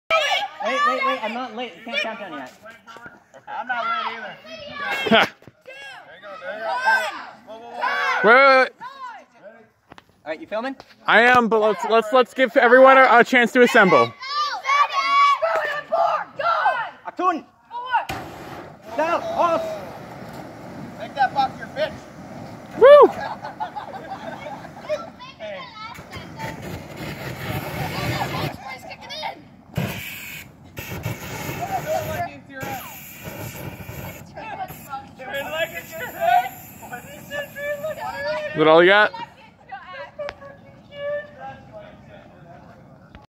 Flamethower second shot
I had some paint thinner left from the first firing.